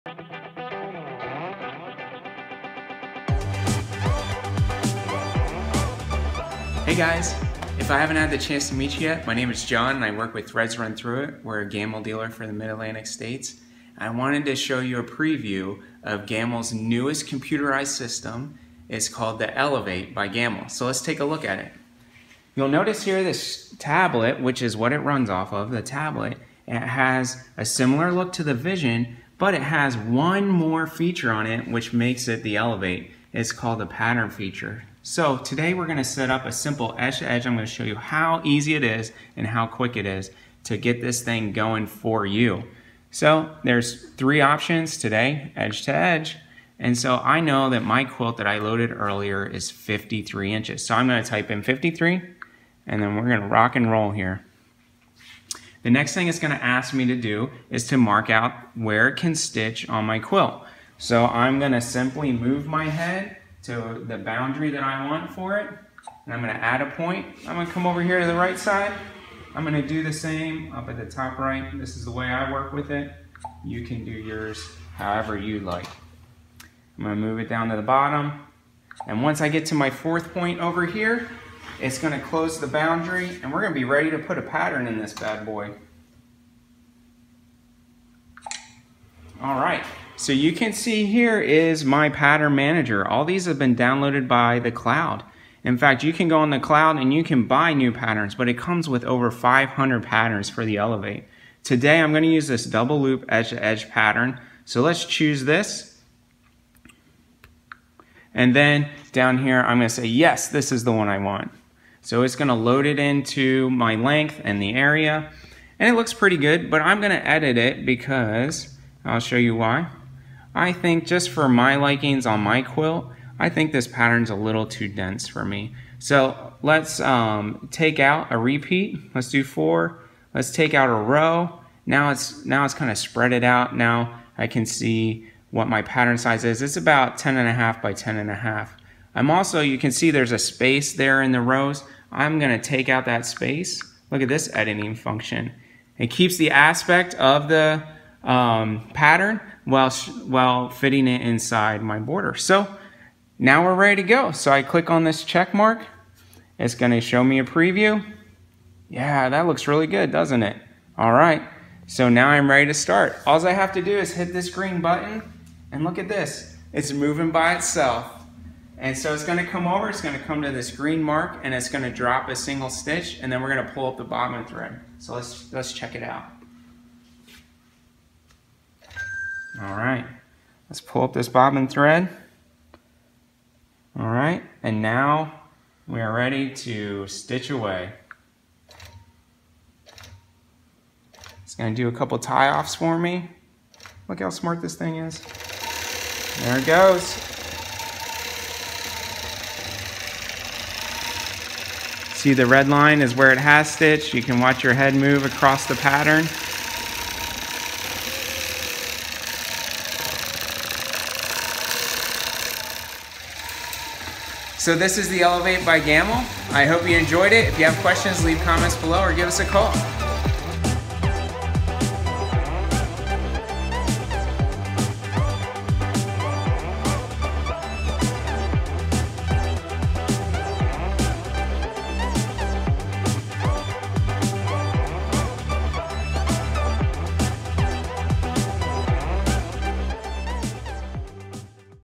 Gammill Elevate: Introduction- Setting Up An Edge to Edge
The Gammill Elevate is an easy to use entry-level computerized quilting system.  Setting up an edge to edge design is very easy and demonstrated in this video.